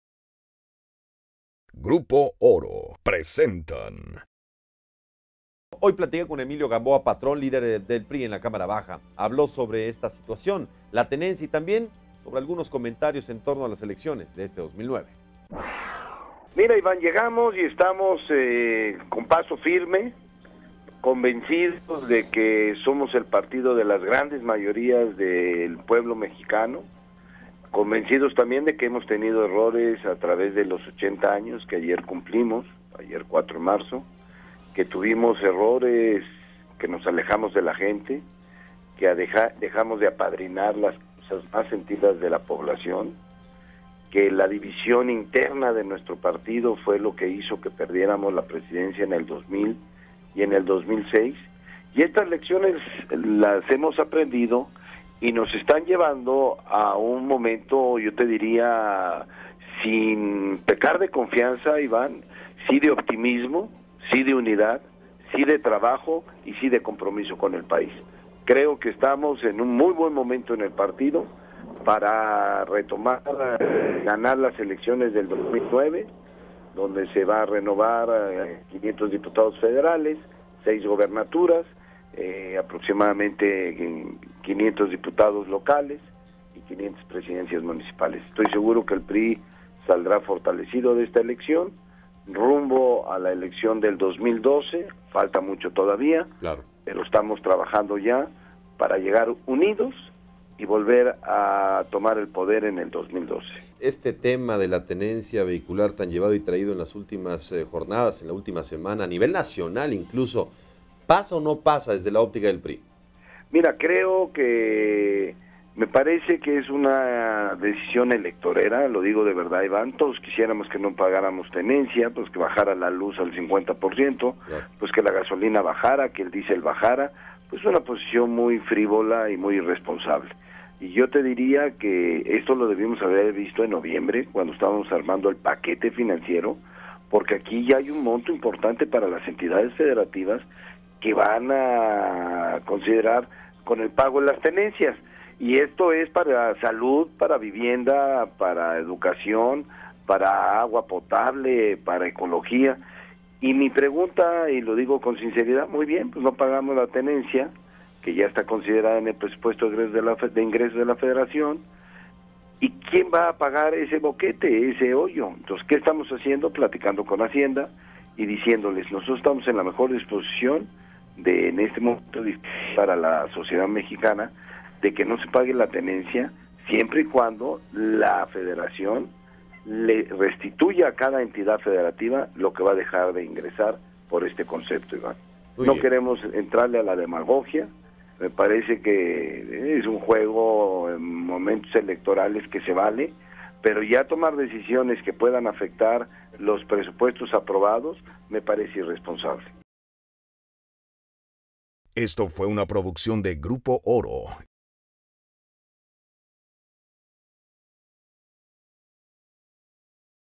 Emilio Gamboa Patrón
Habló sobre la tenencia y los 80 años del PRI